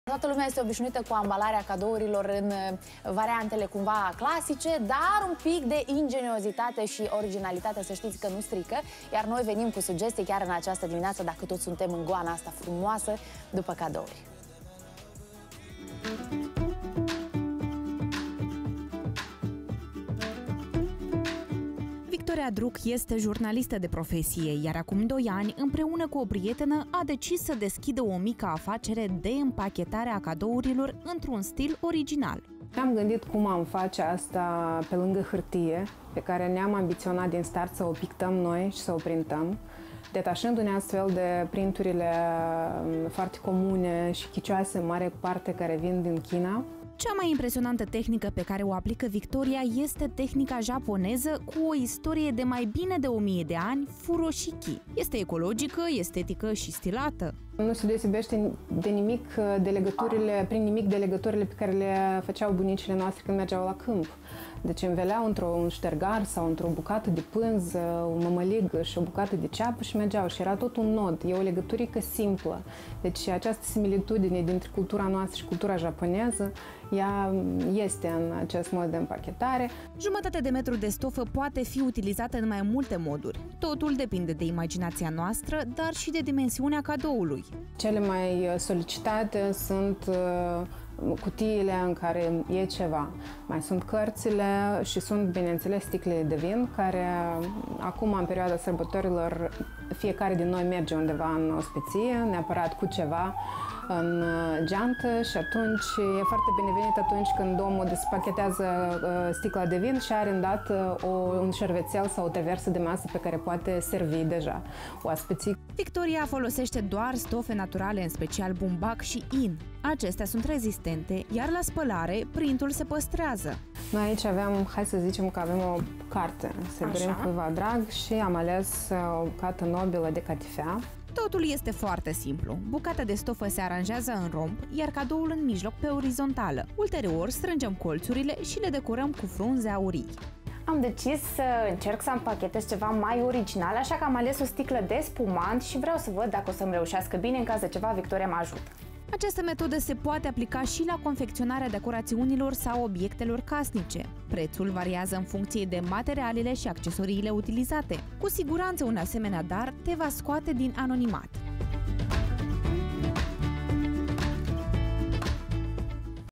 DARURI ÎMPACHETATE ALTFEL
Victoria Druc este jurnalistă de profesie,  iar acum doi ani împreună cu o prietenă a decis să deschisă o mică afacere de împachetare a cadourilor într-un stil original. Cea mai impresionantă tehnică pe care o aplică Victoria este tehnica  japoneză cu o istorie de mai bine de o mie de ani „Furoshiki”.